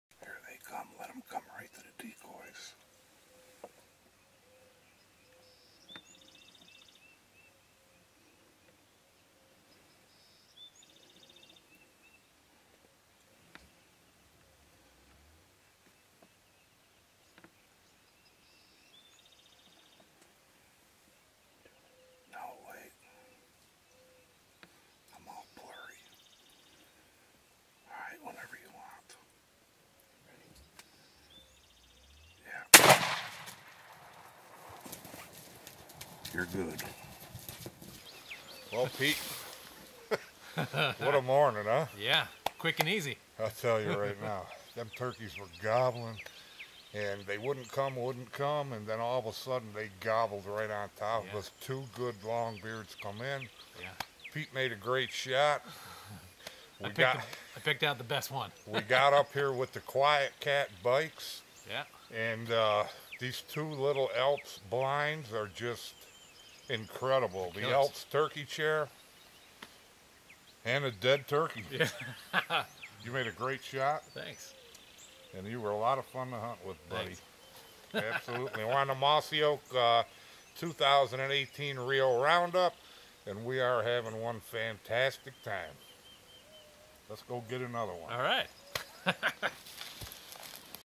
2018 Mossy Oak Rio Round Up - Ernie Calandrelli and Pete
Ernie Calandrelli and Pete from Quiet Kat bag a Rio at the 2018 Mossy Oak Rio Round up at the Ford Ranch in Texas